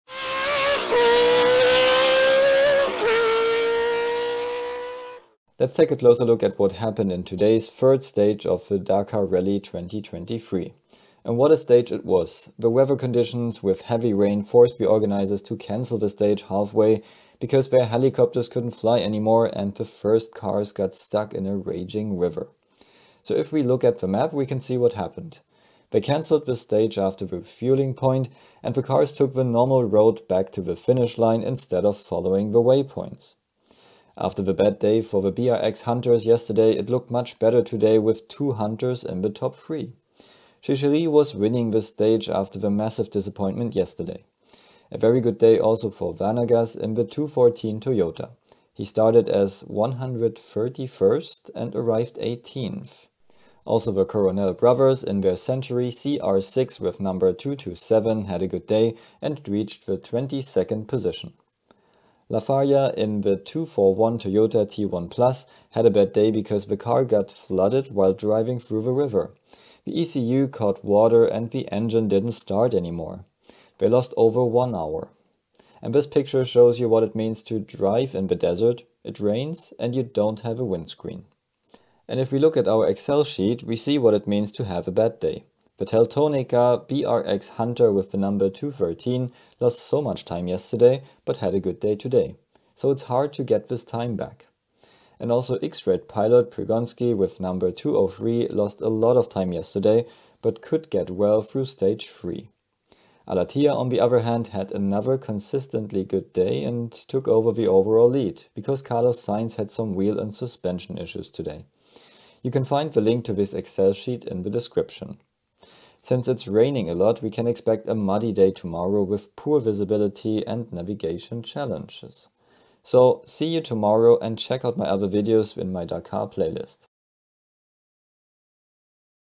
DAKAR Rally 2023 - Stage 3 - Summary
Let's take a closer look at what happened during today's demanding stage 3 of the 2023 Dakar Rally.

Why was the order mixed up today?
Why was the stage cancelled?
What can we expect for tomorrow?

Let me know how you liked stage 3 in the comments below!